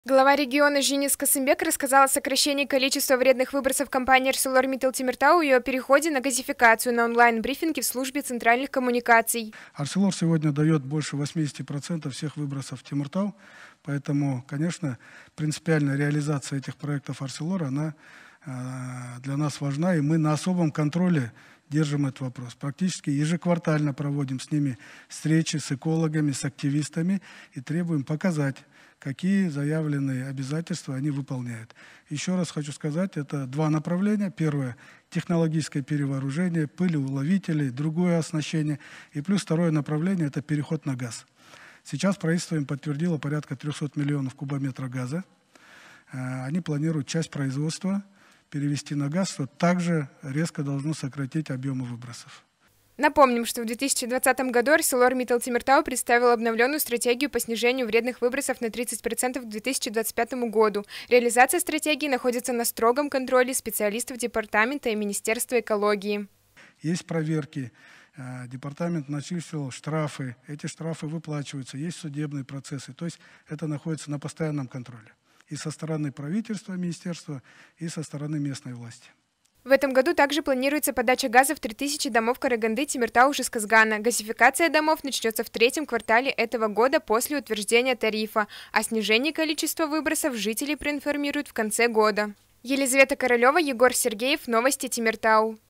Качество воздуха строго контролируется.
Улучшение качества воздуха. Глава региона Женис Касымбек рассказал о сокращении количества вредных выбросов компании «АрселорМиттал Темиртау» и о переходе на газификацию, на онлайн брифинге в Службе центральных коммуникаций.

Женис Касымбек, аким Карагандинской области: ««АрселорМиттал Темиртау» сегодня дает больше 80% всех выбросов в Темиртау.  Поэтому, конечно, принципиальна реализация этих проектов «АрселорМиттал Темиртау». Она для нас важна, и мы на особом контроле держим этот вопрос. Практически ежеквартально проводим с ними встречи: с экологами, с активистами, и требуем показать, какие заявленные обязательства они выполняют. Еще раз хочу сказать, что это два направления. Первое - технологическое перевооружение, пылеуловители, другое оснащение. Второе направление — это переход на газ. Сейчас правительство подтвердило порядка 300 миллионов кубометров газа. Они планируют часть производства перевести на газ, что также резко должно сократить объемы выбросов.».

Напомним, что в 2020 году «АрселорМиттал Темиртау» представил обновленную стратегию по снижению вредных выбросов на 30% к 2025 году. Реализация стратегии находится на строгом контроле специалистов департамента и министерства экологии.

Женис Касымбек, аким Карагандинской области: «Есть проверки. Департамент начислил штрафы. Эти штрафы выплачиваются. Есть судебные процессы. То есть, это находится на постоянном контроле. И со стороны правительства Министерства, и со стороны местной власти».

В этом году также планируется подача газа в 3 тысячи домов Караганды, Темиртау и Жезказгана. Газификация домов начнется в третьем квартале этого года после утверждения тарифа. О снижении количества выбросов жителей проинформируют в конце года.